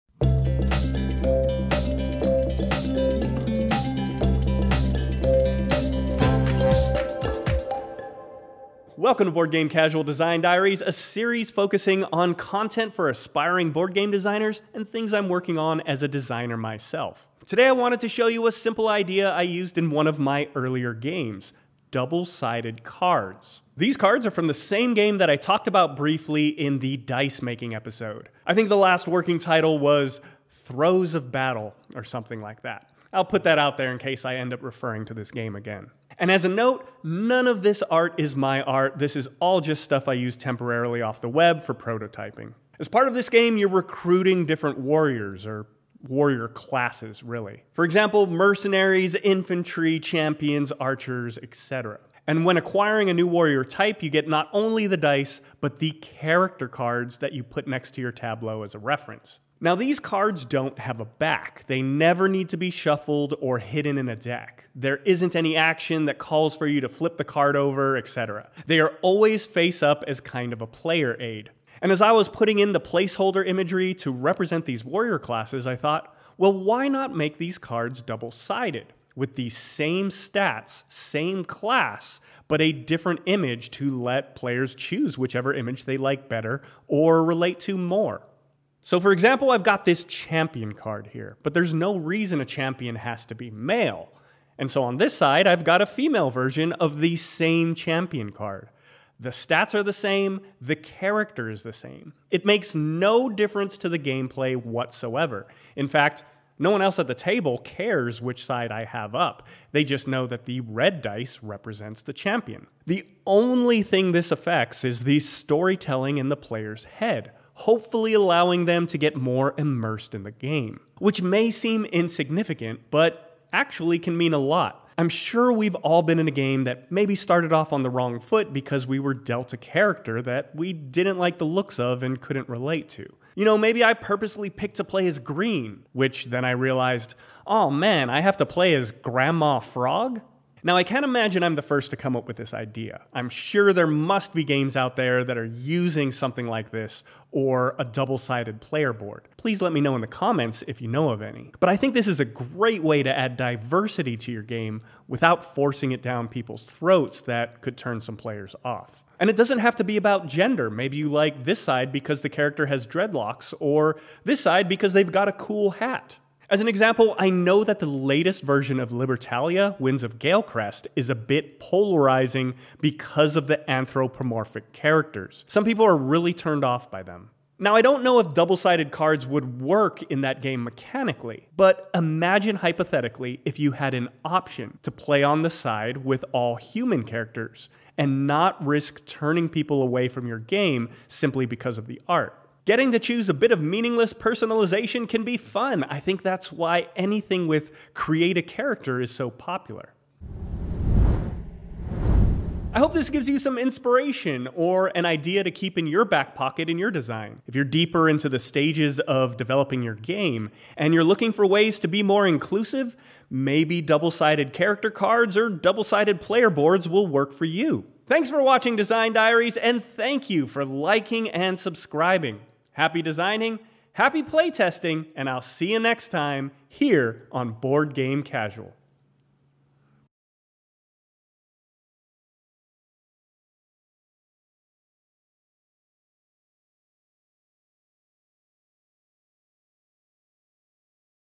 Double-sided Cards: An easy way to add diversity and inclusivity to your game! | Design Diaries Ep 5
Design Diaries Episode 5:  I'll show you an idea I had for double-sided cards in one of my game designs, which is an easy way to add more diversity or inclusivity to your game.

00:00 Intro
00:42 Game Context
01:11 Double-sided Cards
02:04 Why This Matters
02:31 Add Diversity
03:21 Final Thoughts

Board Game Casual:
BGG/Reddit: Cardboard_RJ
Camera: iPhone 14 Pro
Mic: Zoom H2n